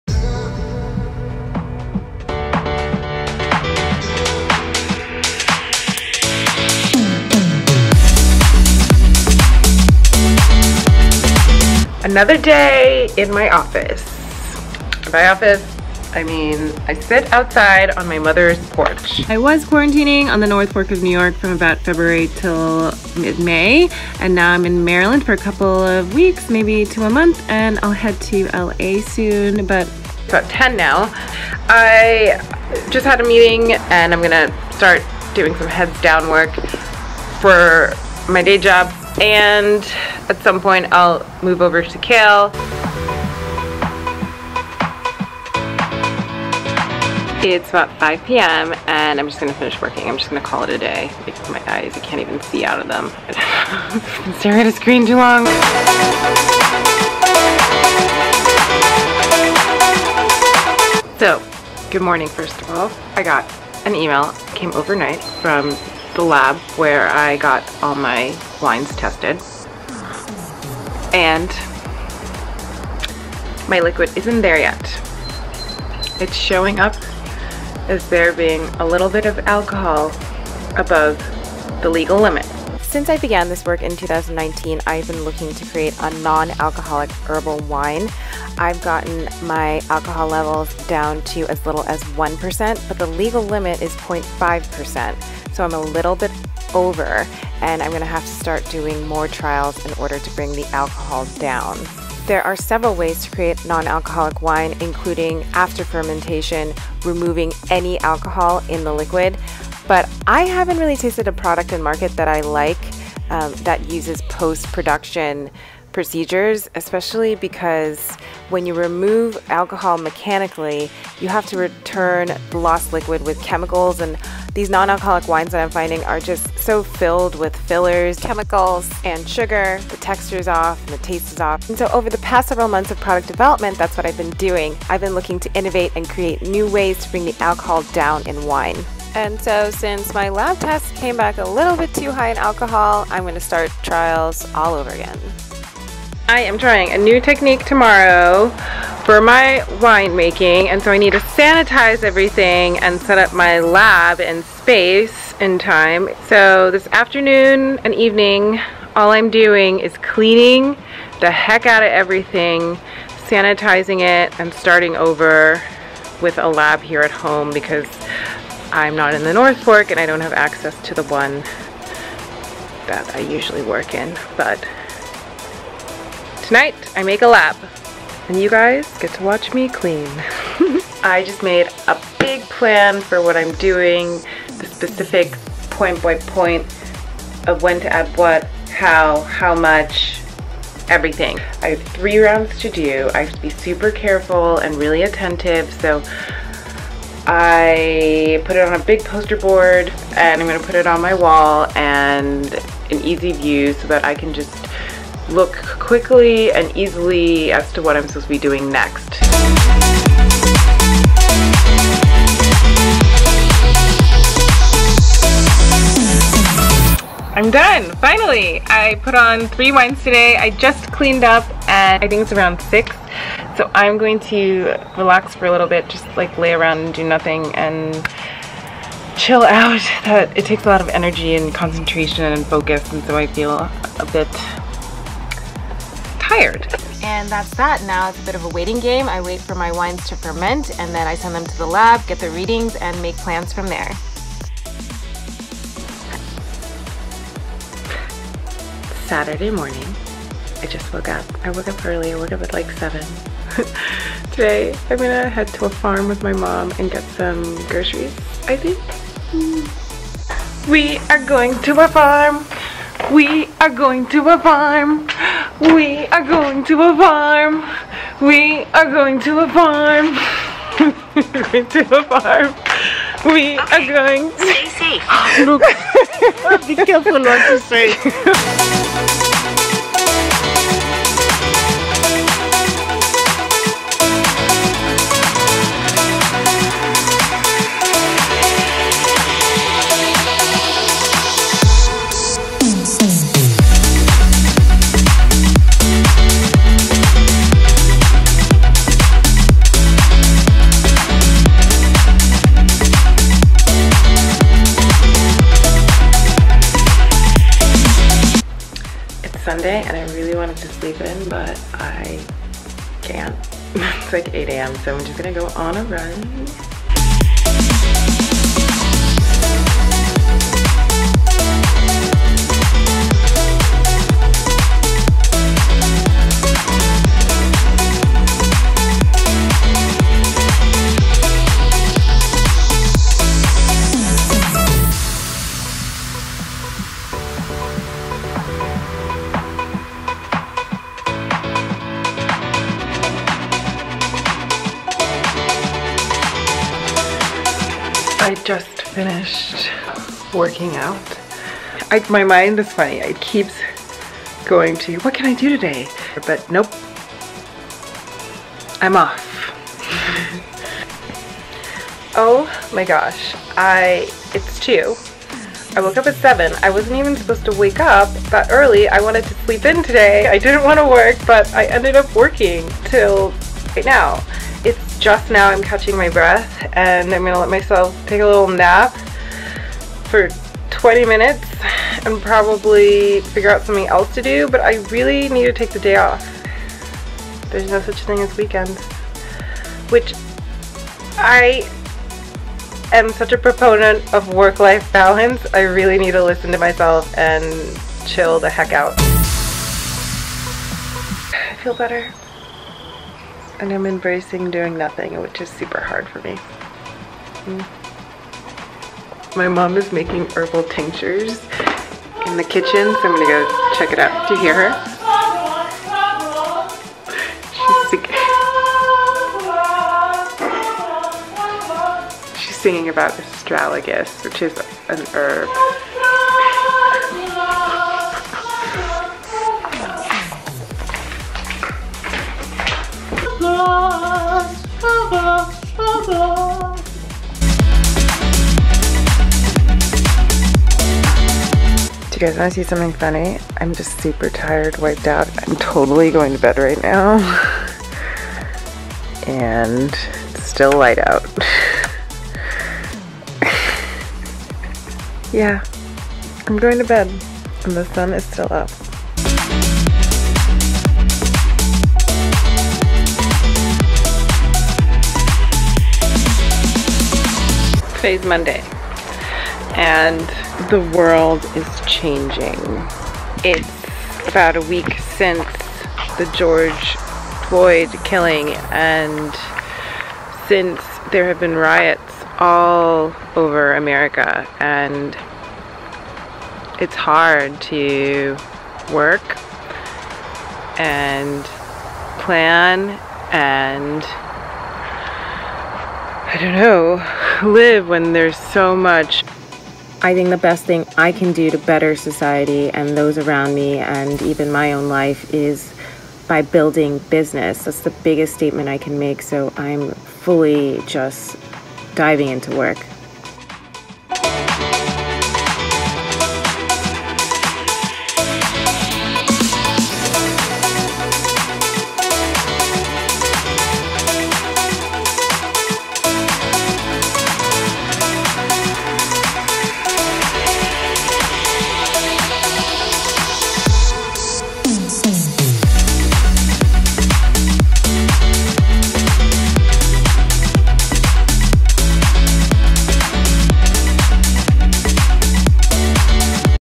Day In the Life of a Startup Entrepreneur | Ep 21| Wine Making
Filmed in May and June 2020.

My name is Nana and I am founder of Cale - a wellness company focused on alcohol alternatives. My first release will be a line of low alcoholic wines infused with healthful herbs, botanicals, plants and adaptogenic mushrooms: Herbal Wine.

I first got the idea at the turn of the year December 2018 to January 2019. I began filming my progress in the fall of October 2019 to help inspire others - I hope my startup journey will help you along yours or whatever it is you are choosing to build and aspire to.

This is Episode 21, watch as I continue to build my wine company.